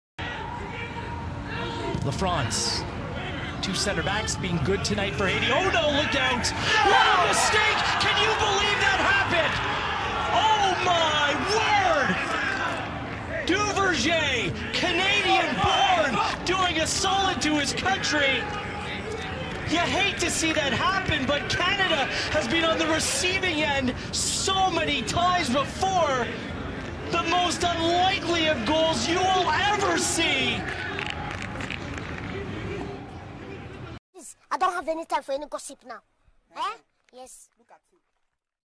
Is this one of the greatest own goals of all-time? 😱 FIFA world cup 2022 Qualifier Canada vs Haiti.
In the CanadaVsHaiti match, Haiti Goalkeeper Josué Duverger lived out every netminder’s worst nightmare after he failed to control a pass from his left-back.
Canada reached the final round of the CONCACAFWorld Cup qualifiers after Haiti goalie Josué Duverger made one of the biggest blunders of his career which has now been dubbed as the 'worst own goal in football history'. In the CanadaVsHaiti match, Haiti Goalkeeper Josué Duverger lived out every netminder’s worst nightmare after he failed to control a pass from his left-back. Visibly panicking, Duverger in his last-ditch attempt accident gave more leverage to the ball with his left foot and drove it straight to the net.